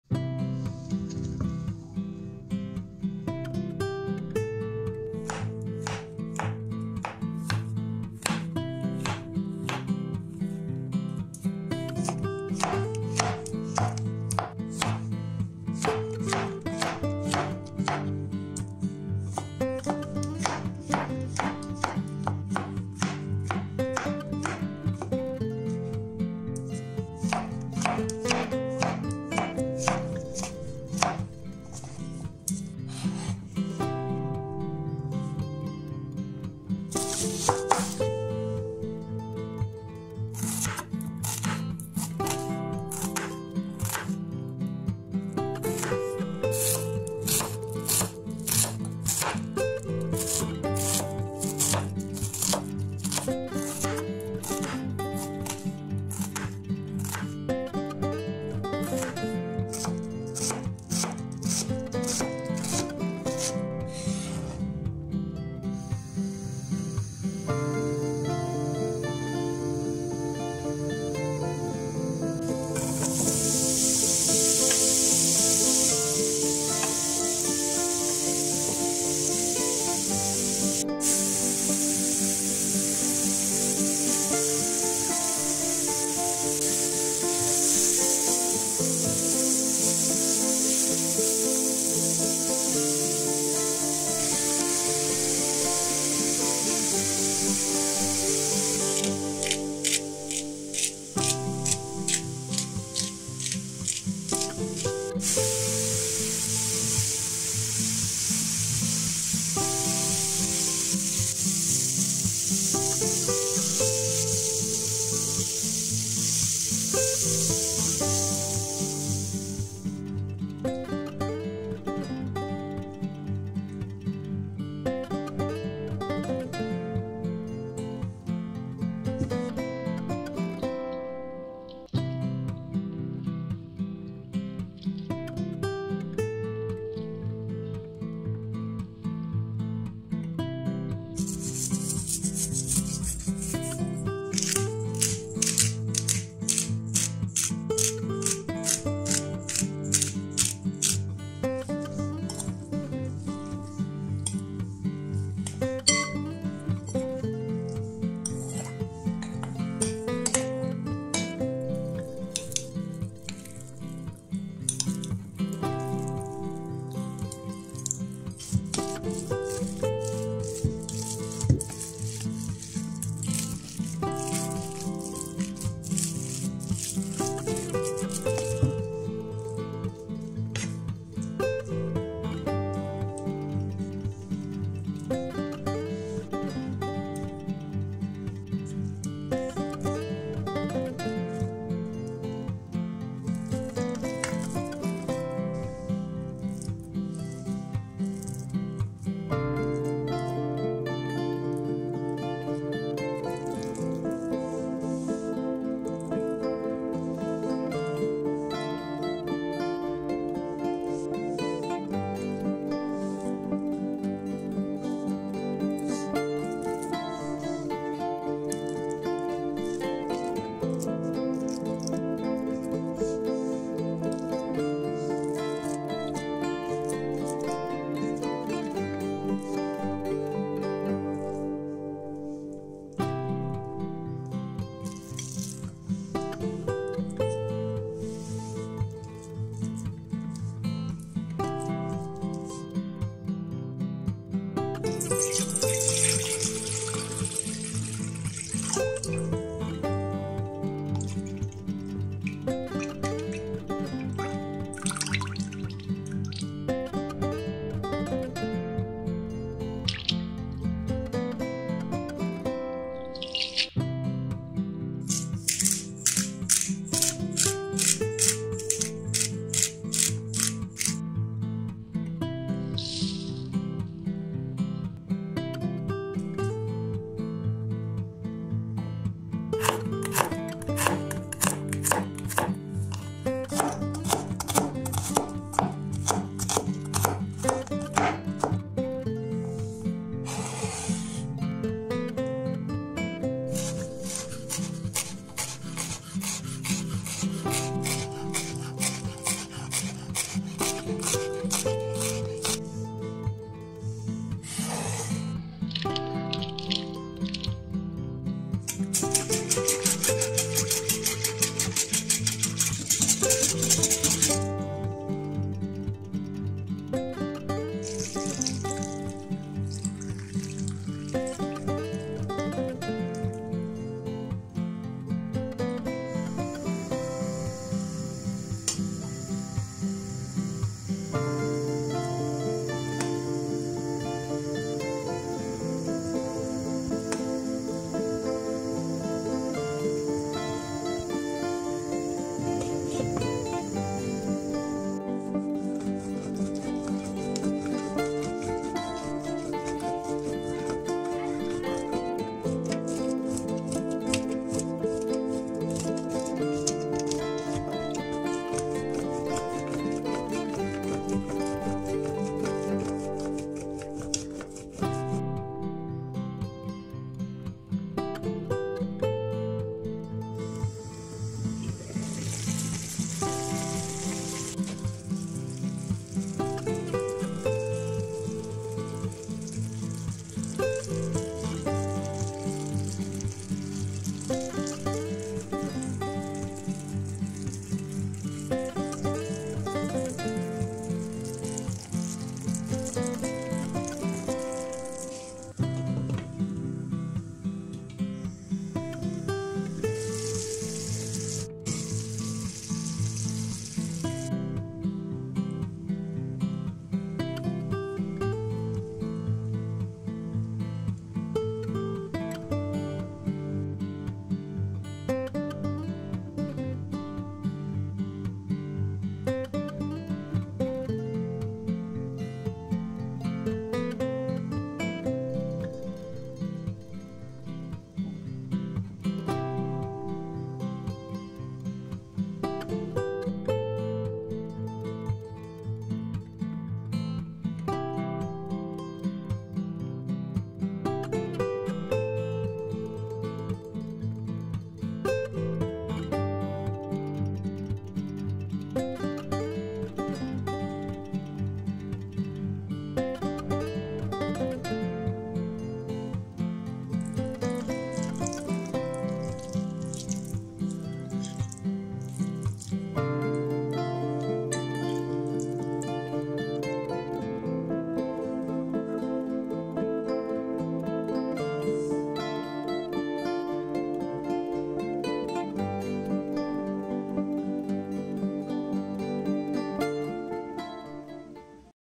Es ist sehr lecker. Ich koche jedes Mal, wenn ich Gäste habe.
Ein sehr ungewöhnliches Rezept. Jedes Mal, wenn ich es koche, sind meine Gäste begeistert. Sehr leckere Zutaten und einfach zuzubereiten. Sie benötigen: Nudeln, Putenhackfleisch, Sahne. Alles sehr lecker und einfach. Ein wenig Geschick beim Kochen und alle werden glücklich sein. Bereiten Sie sich auf die Gesundheit vor!

Rezept und Zubereitung:

200 g Champignons
1 Zwiebel
Pflanzenfett
Salz Pfeffer
200 g Putenhackfleisch
Tagliatelle-Nudeln
300 ml Wasser
200ml Sahne
1 Teelöffel Dijon-Senf
1 Teelöffel Kräuter der Provence
1 Teelöffel Mehl
1 Knoblauchzehe
Dill
100 g Käse

Guten Appetit!

👍 Gefällt mir!
🔔 Schlagt die Glocke), um das neue Video nicht zu verpassen!